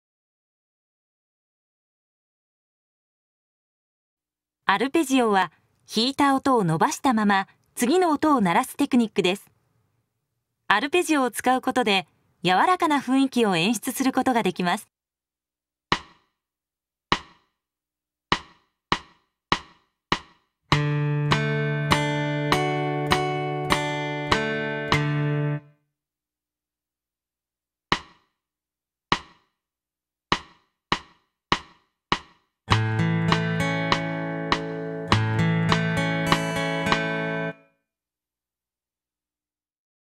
07 アルペジオで弾いてみよう！
『DVDで今日から弾ける！かんたんアコースティック・ギター New Edition』
および
 『DVD&CDでよくわかる！ はじめてのアコースティック・ギター New Edition』
の連動動画です。